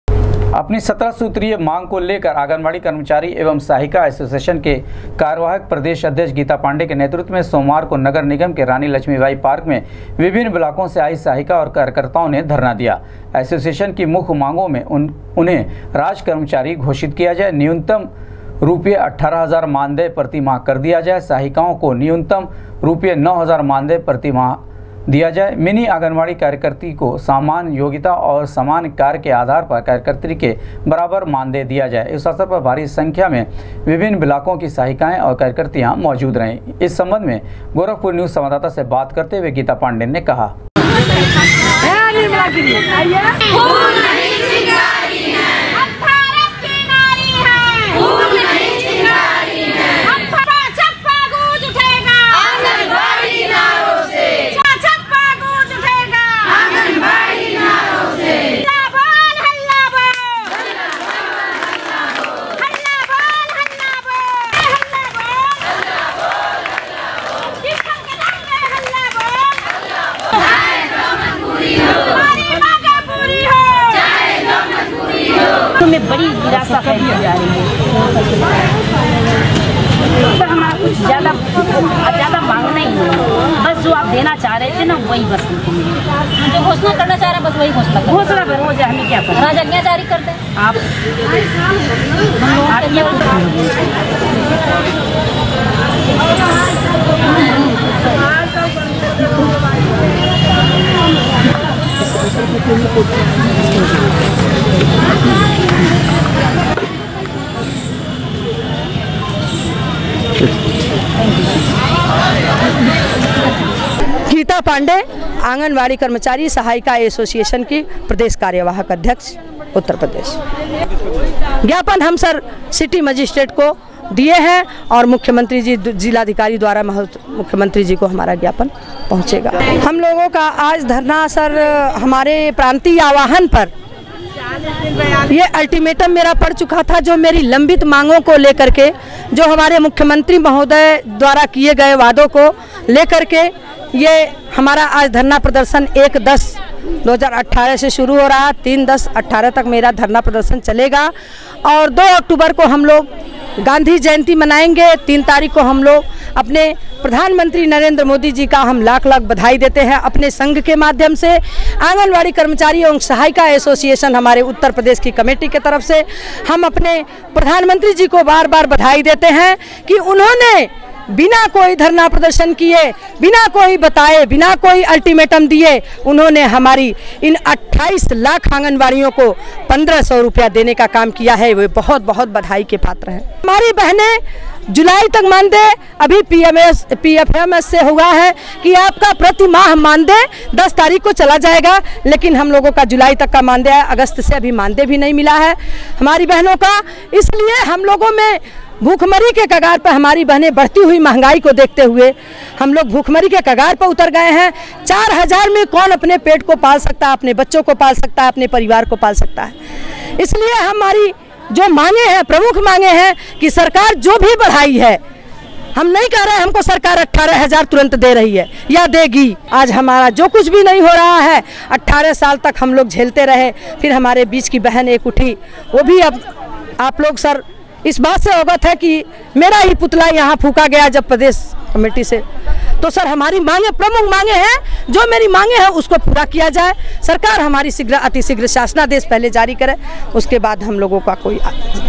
विभिन्न मांगों को लेकर आंगनबाड़ी कर्मचारी एवं सहायिका एसोसिएशन ने दिया धरना, जिला अधिकारी
विभिन्न मांगों को लेकर आंगनबाड़ी कर्मचारी एवं सहायिका एसोसिएशन ने दिया धरना, जिला अधिकारी के माध्यम से मुख्यमंत्री को सौंपा ज्ञापन